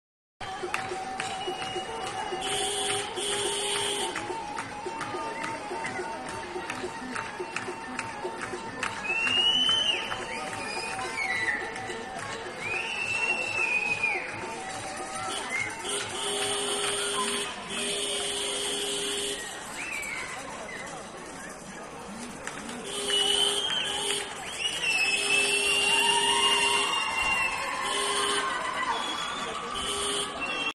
Bursa'da 'gelin alma' rezaleti! Dakikalarca sürdü...
Bursa'da bir sokakta 'gelin alma' töreninde halay çeken onlarca kişi sosyal mesafe kuralını hiçe saydı.